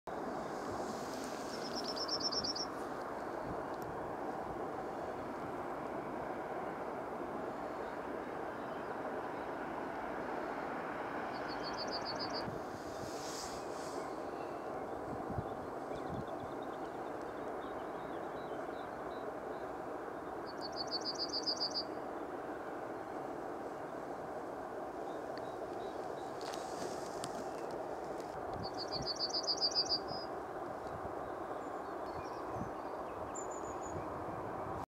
Yellowhammer Hartshead singing.mov
Yellowhammer singing away above a busy main road at Hartshead,UK.